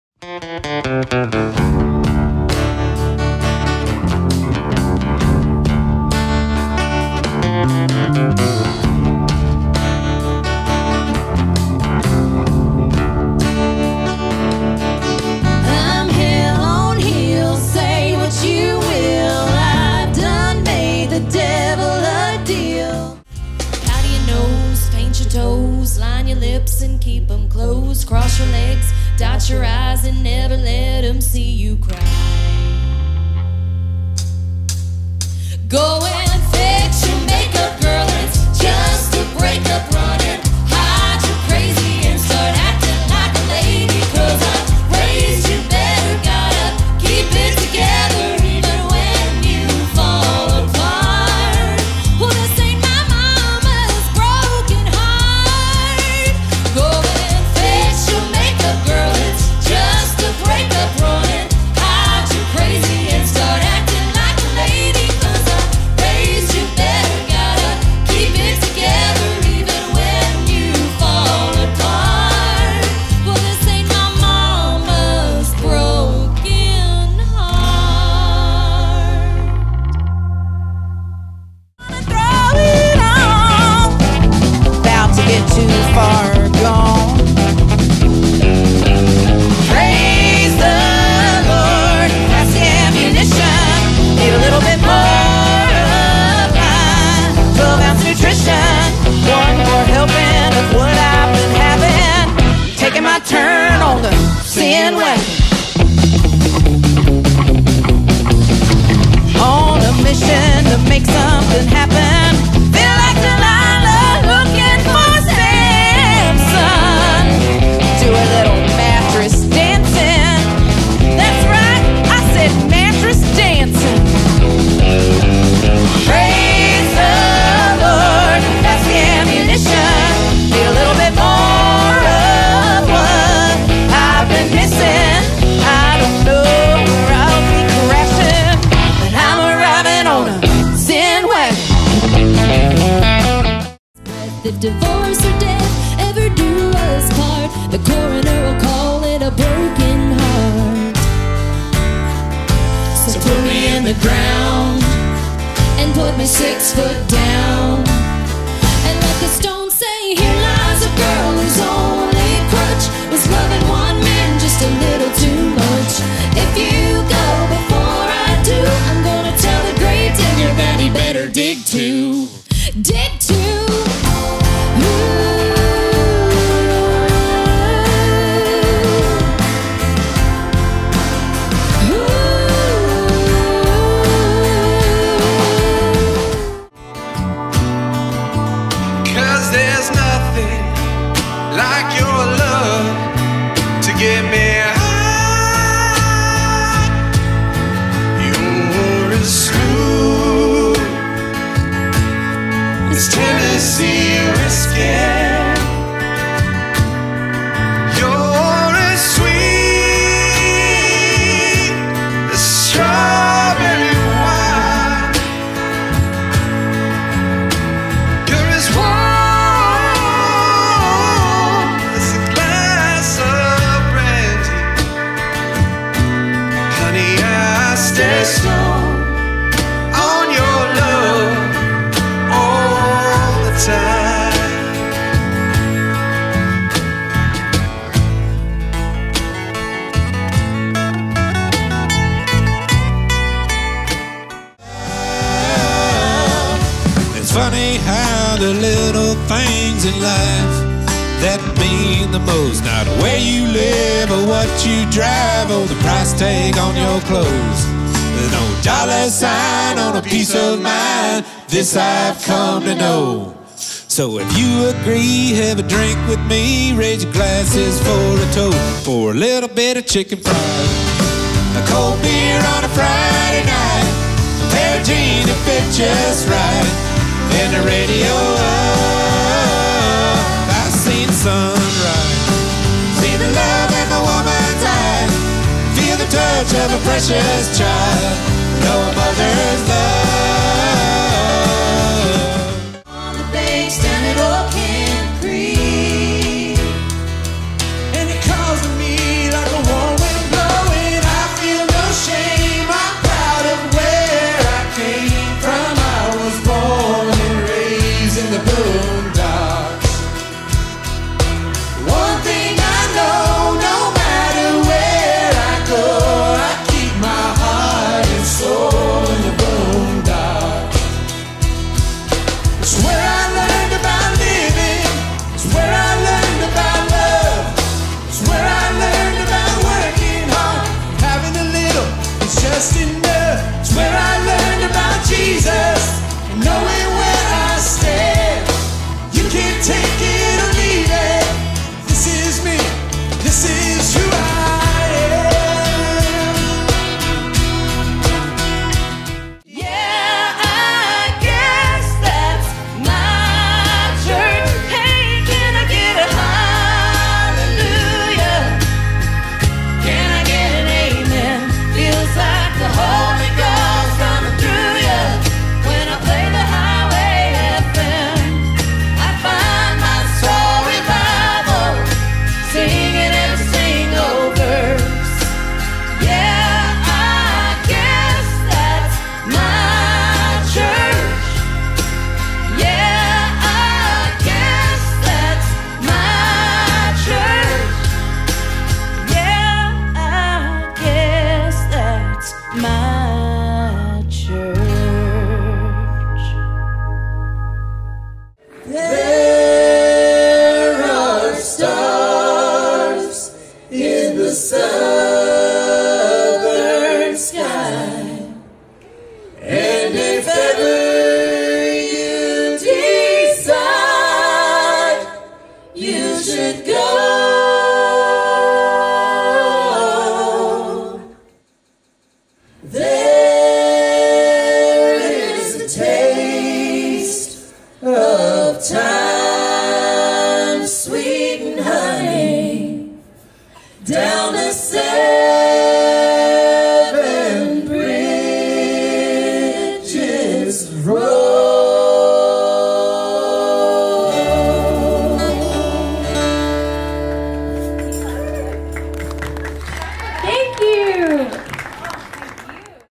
Alton Bridge Promotional
Alton Bridge is a successful local cover band for some of the best country music out there. Watch some of their demos in this Fall promotional.